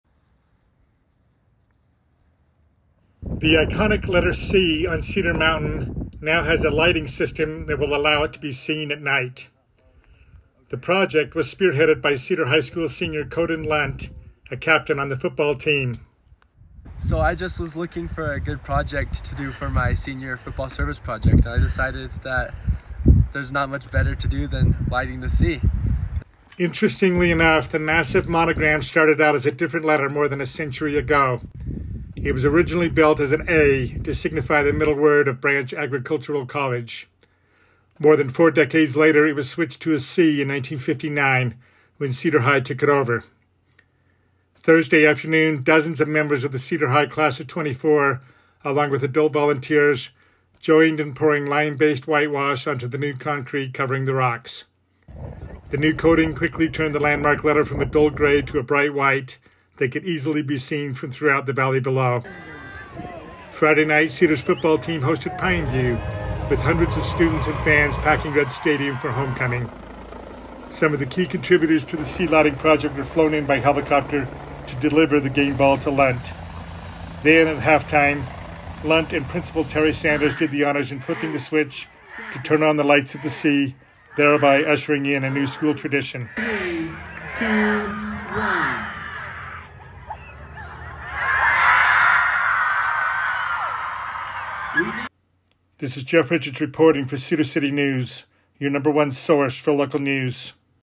Cedar High seniors, supporters join in bringing lights to iconic ‘C’ on Cedar Mountain
CEDAR CITY — The iconic letter “C” on Cedar Mountain was lit up for the first time on Friday night, during Cedar High School’s homecoming football game.
Football team captain Koden Lunt joined principal Terri Sanders at midfield at halftime as the crowd loudly counted down to zero.
As Lunt pushed down on a large cardboard plunger that was just for show, Sanders activated the actual switch using a phone app, thereby turning on the 70 recently installed lights that now outline the “C”.